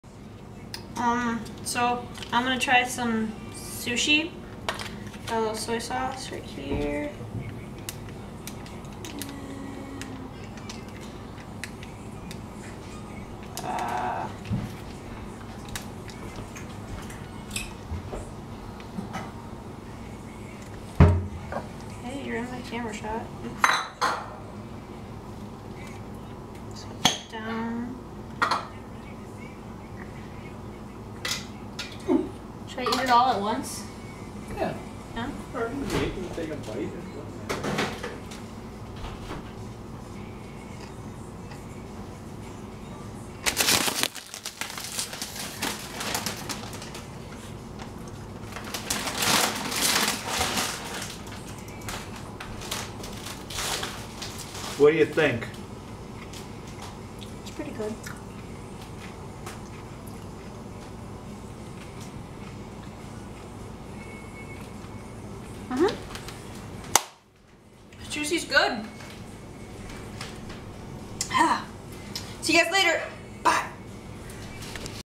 Sushi for the first time
I try sushie for the first time in my life!!!
 I had a california roll with some soy suace on top and it was really good! And it took so long to opne the soy sauce because i didn't want it to like explode onto me!